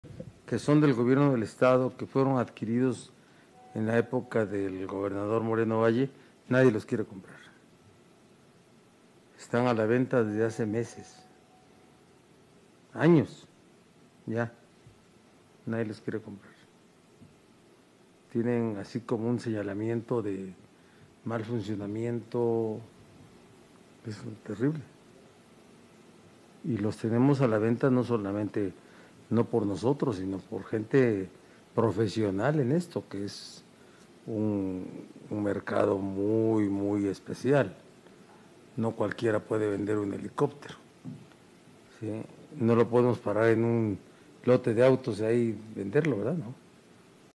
Nadie quiere comprar los helicópteros adquiridos durante administraciones pasadas
El gobernador de Puebla, @LMiguelBarbosa , aseguró que nadie quiere comprar los helicópteros adquiridos durante la administración del ex mandatario Rafael Moreno Valle, ya que están señalados por tener un "mal funcionamiento".